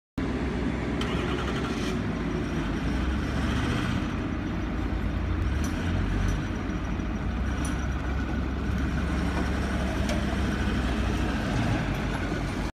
2008 FORD F550 SD For Sale
17950# GVWR, 6.4 Cyl Powerstroke Diesel,  Automatic, S/N: 1FDAX56R28ED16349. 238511 Miles  Indicated.

 [FORD F550 EXTENDED CAB SERVICE TRUCK,]